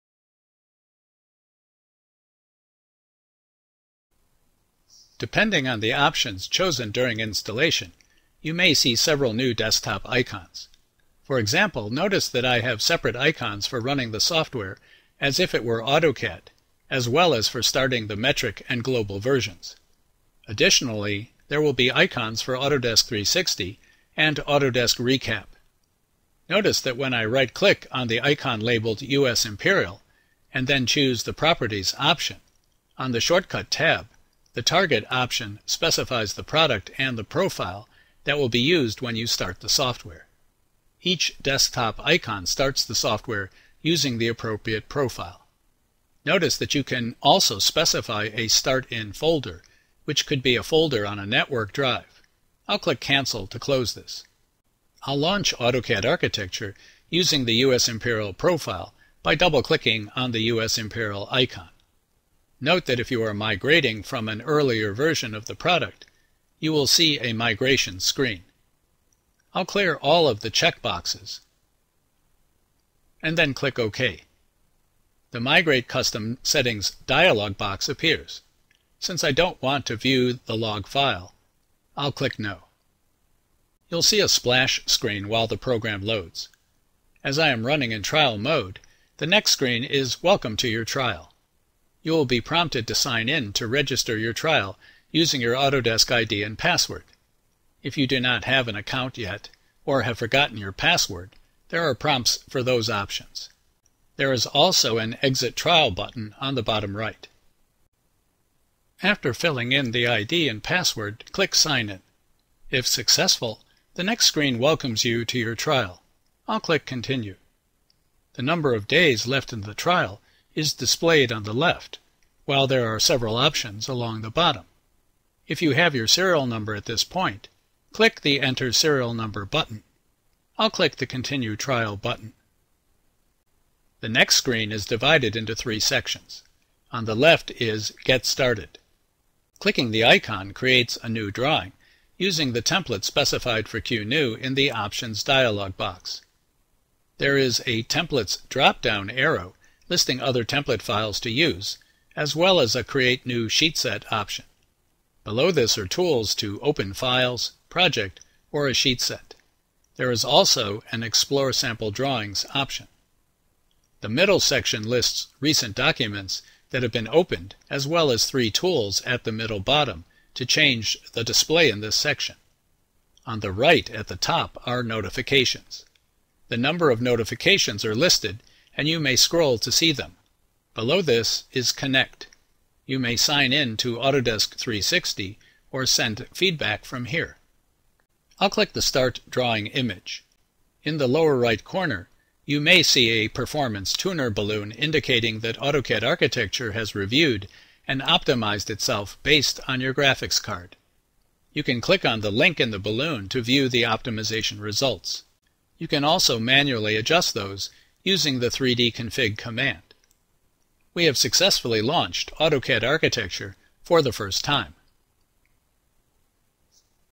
Launching the Application
Walking through the desktop icons, profiles, and software authorization for AutoCAD Architecture 2015 .